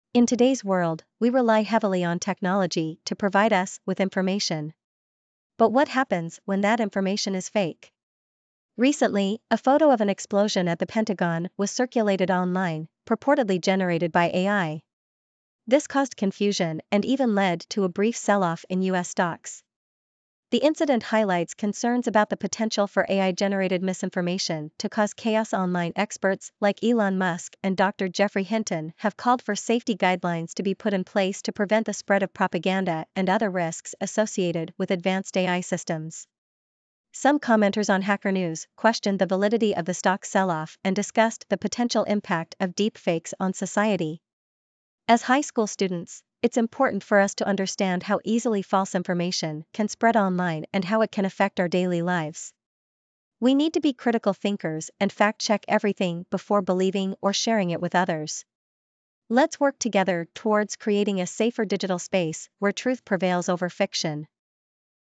AI-Generated Fake Pentagon Explosion Photo Causes Chaos & Stock Selloff
A fake photo of an explosion at the Pentagon, purportedly generated by AI, caused confusion on social media and briefly led to a selloff in US stocks. The incident highlights concerns about the potential for AI-generated misinformation to cause chaos online. Experts, including Elon Musk and Dr. Geoffrey Hinton, have called for safety guidelines to be put in place to prevent the spread of propaganda and other risks associated with advanced AI systems. Some commenters on the HackerNews post questioned the validity of the stock selloff and discussed the potential impact of deepfakes on society.